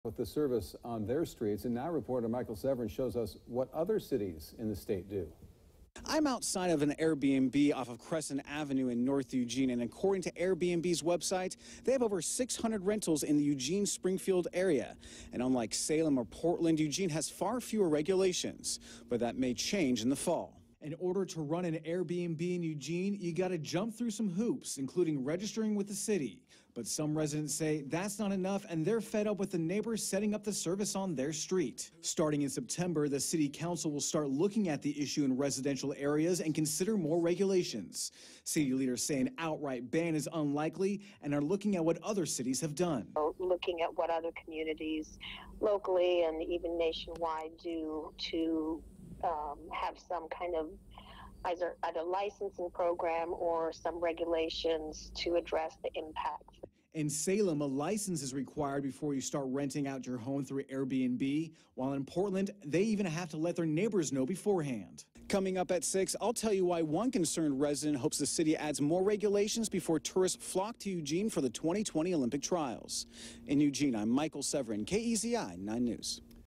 Eugene considers adding more regulations for Airbnb in residential areas
Eugene is looking into possible regulations for short-term rentals in residential areas, according to Building and Permit Services Director Lydia Kaye.